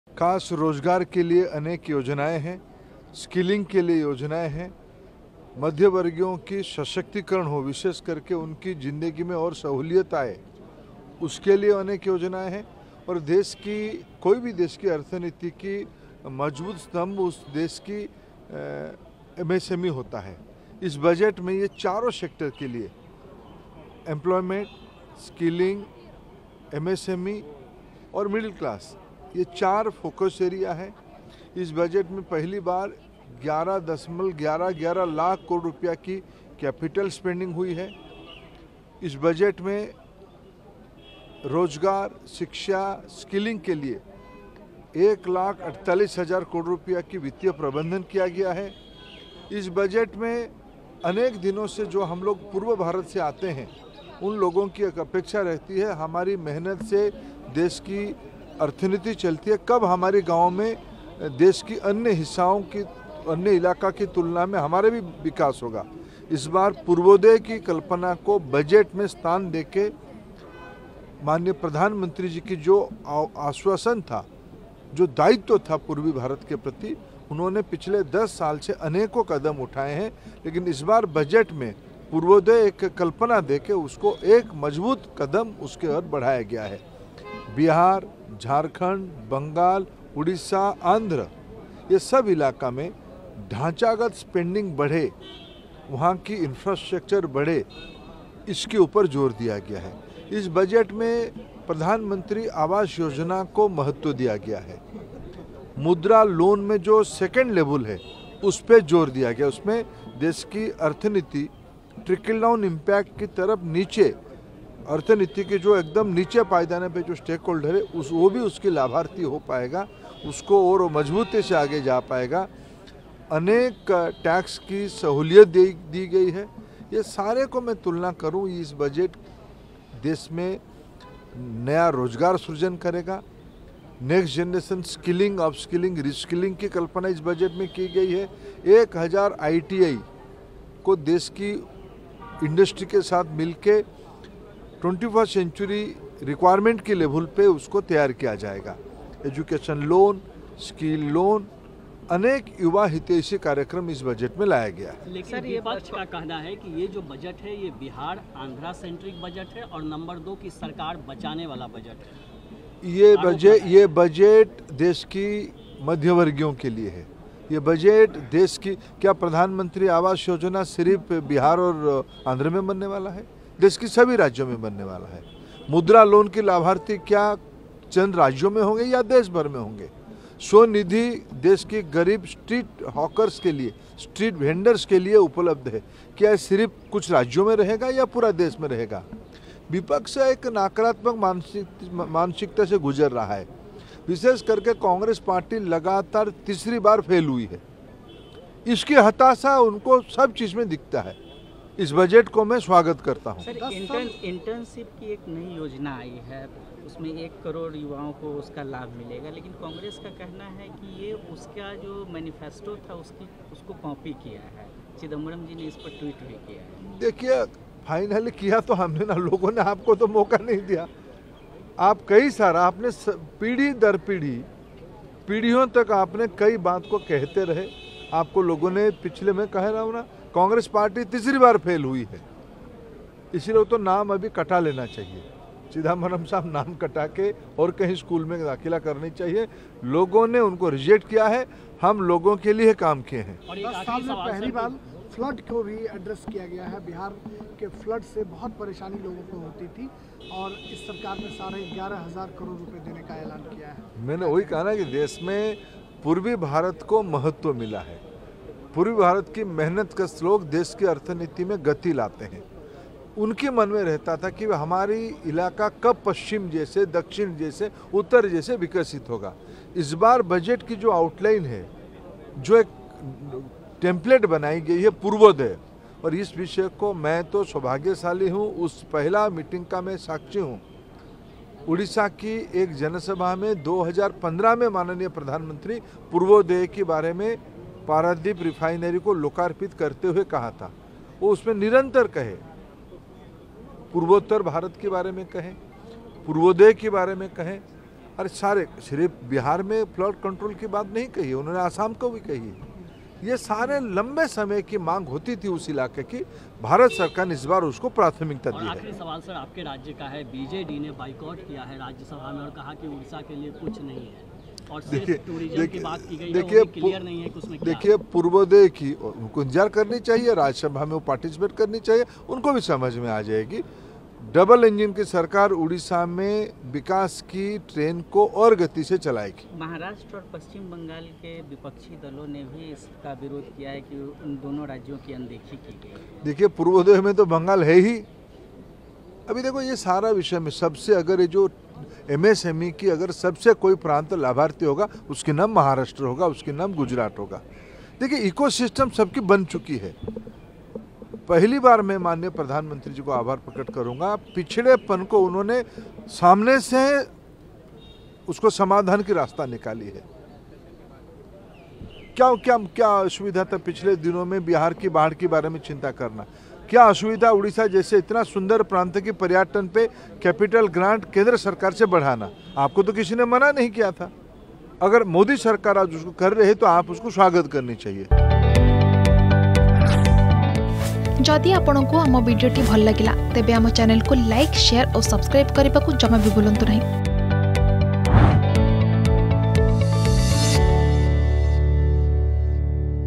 ଏ ବଜେଟ୍ ମଧ୍ୟବର୍ଗୀୟ ସଶକ୍ତିକରଣର ବଜେଟ || Dharmendra Pradhan || Budget
ଏ ବଜେଟ୍ ମଧ୍ୟବର୍ଗୀୟ ସଶକ୍ତିକରଣର ବଜେଟ : ଧର୍ମେନ୍ଦ୍ର ପ୍ରଧାନ, କେନ୍ଦ୍ର ଶିକ୍ଷାମନ୍ତ୍ରୀ